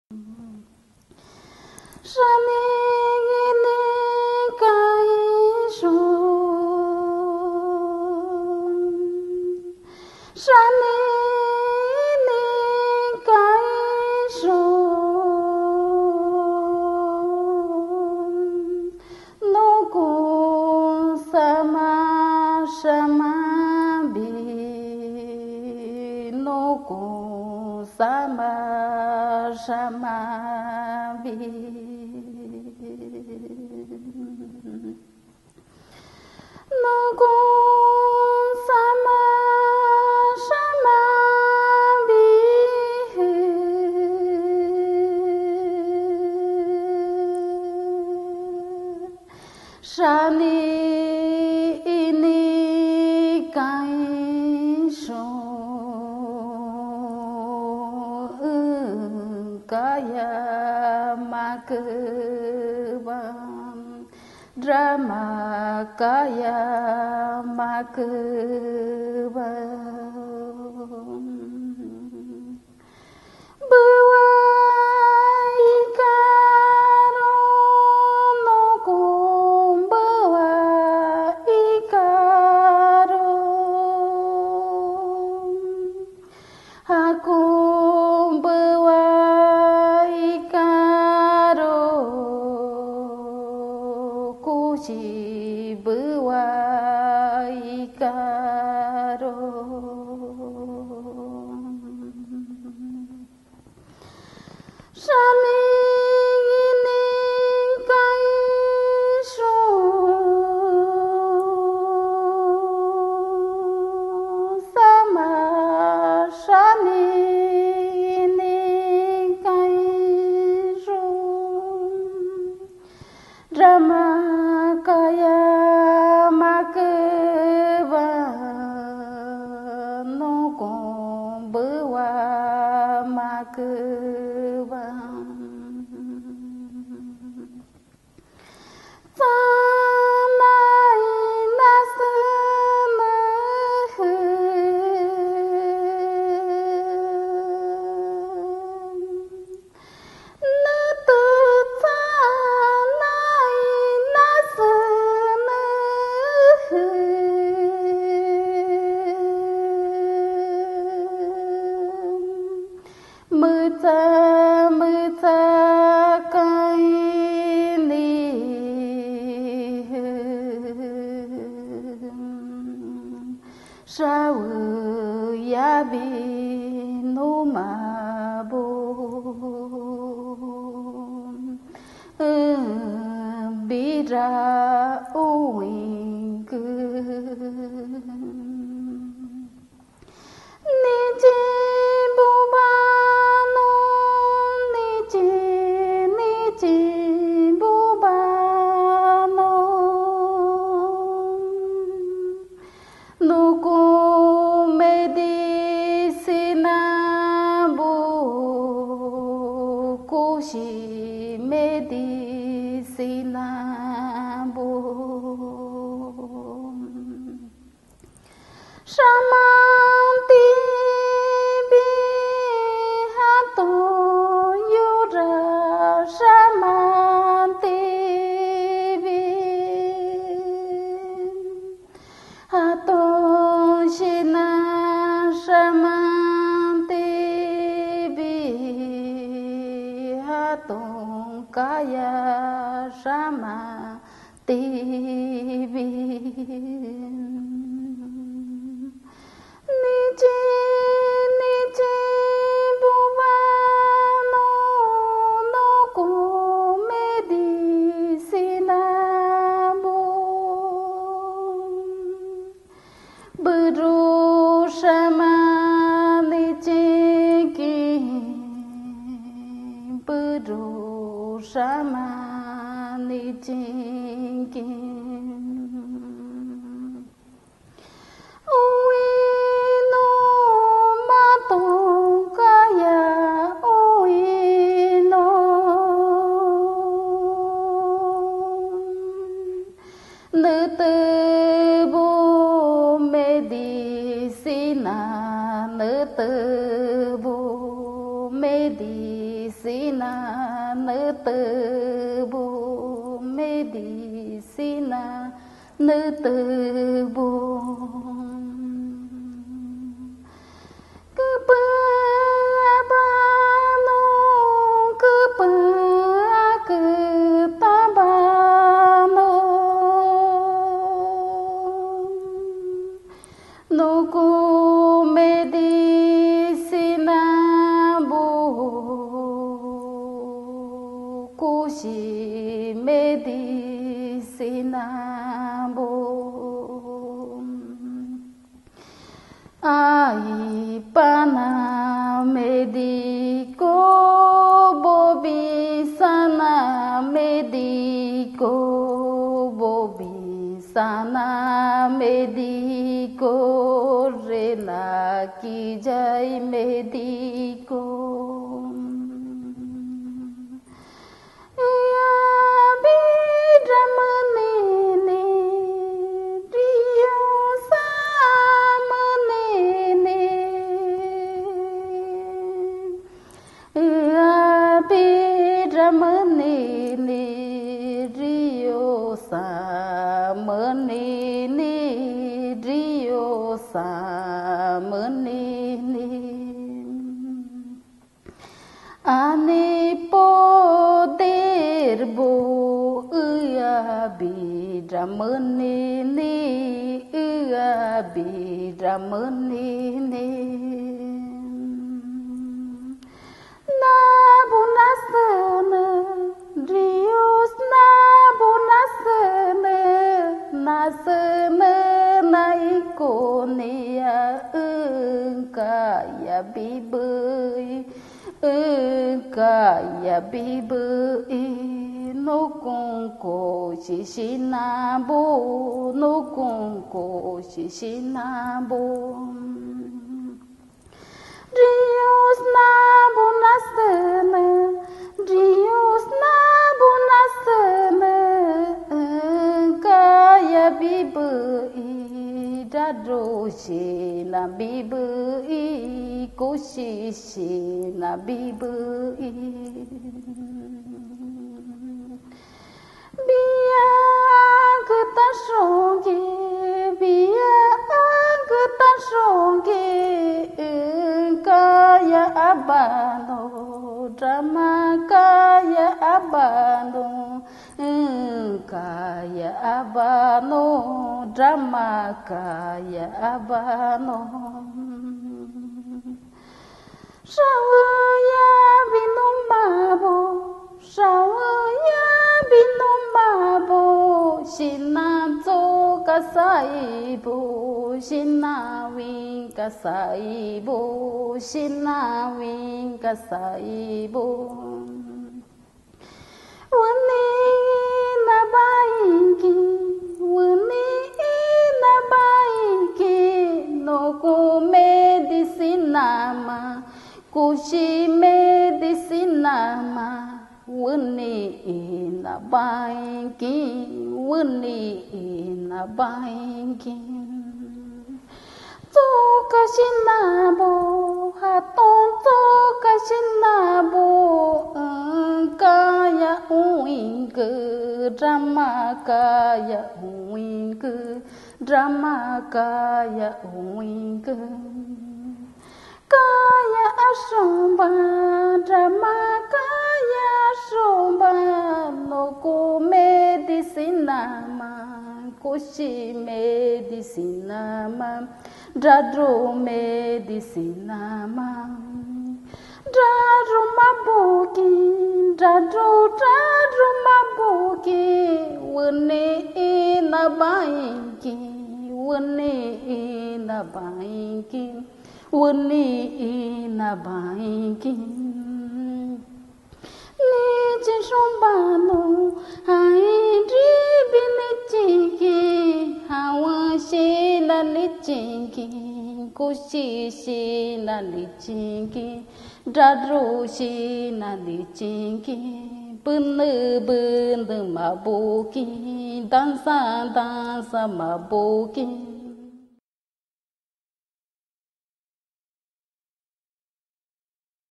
Icaro Ayahuasca
Canto de la Ceremonia de Ayahuasca...Doña Karina Garcia Rios